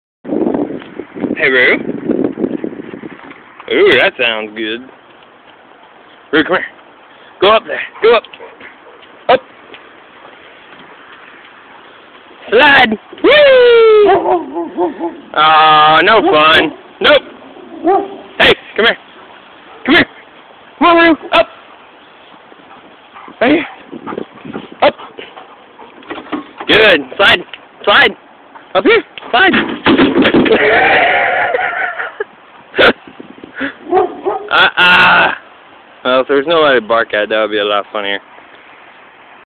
Presa Playground Cred 2
Sorry for the crappy video.  I forgot I changed the record settings.  BOO!

Here is another video of my Presa Canario acting a fool at the park.

She whines when we walk by the park and don't go in.  I try to let her slide at least once.

If there wasn't a person to bark at, "HEY! YOU DIDN'T PET ME!" She would have ran like crazy like at the beginning of the first video.